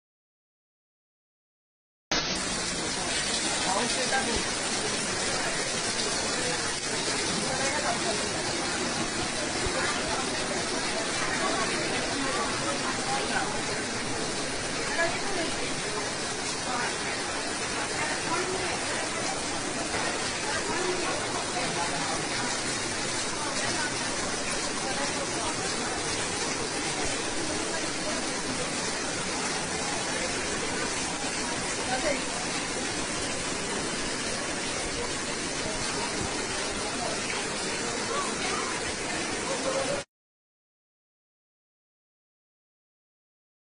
Statue of Unity India
Rainwater seeps into viewing gallery of Statue of Unity, netizens slam it as ‘unfortunate’ Statue of Unity’s Viewing Gallery. This is the situation after one rain, it started leaking, the corridor is flooded...